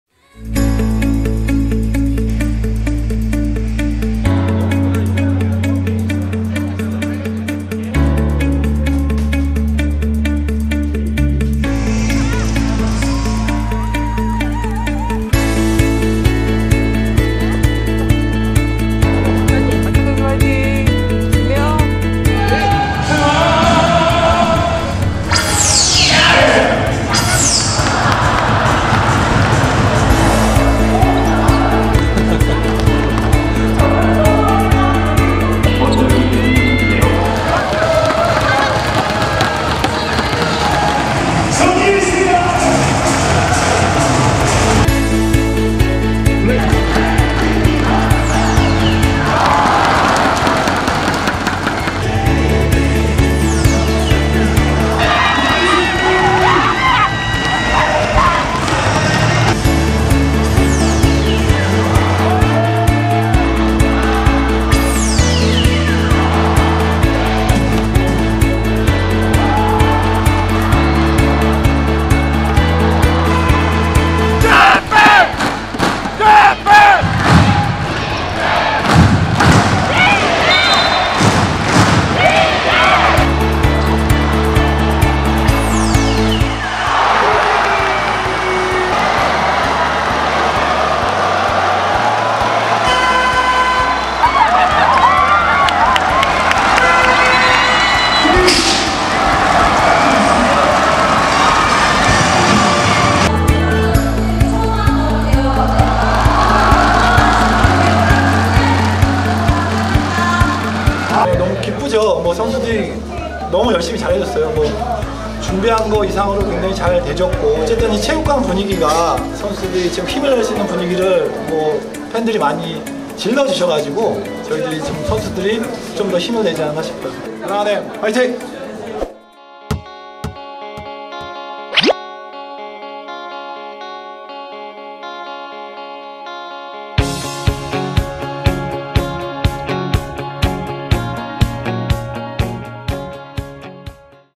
[WKBL 개막전] KEB하나은행 VS BNK썸 역대급 경기가 펼쳐졌다!!
10월 19일 WKBL 개막일!

부천 KEB하나은행 실내체육관에서
KEB하나은행과 BNK썸의 맞대결로 경기가 펼쳐졌는데요

신흥 라이벌로 핫한 두 팀의 뜨거운 열기를
저희 제작진이 담아왔습니다!

역대급 개막전 경기가 펼쳐진 현장!
함께 보실까요?

재밌게 보셨다면
구독👍🏻 좋아요💗
많이 눌러주세요